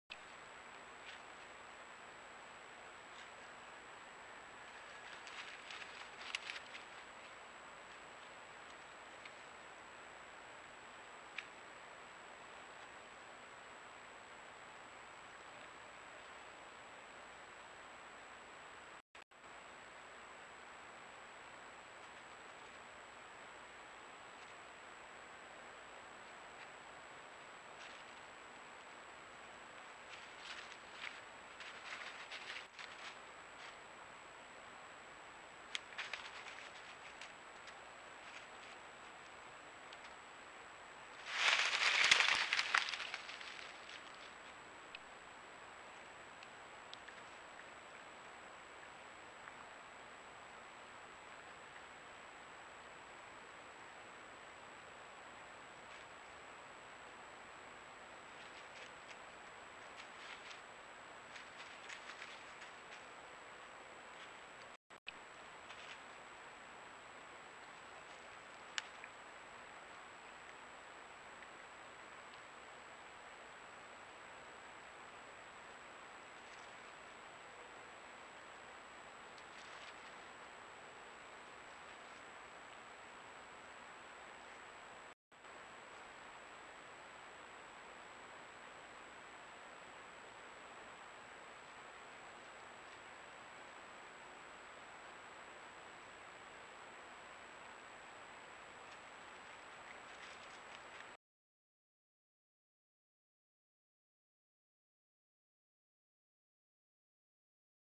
Timid Fox Snatches the Chicken Carcass - Browning Spec Ops Advantage Trail Cam
Caught with the Browning Spec Ops Advantage trail cam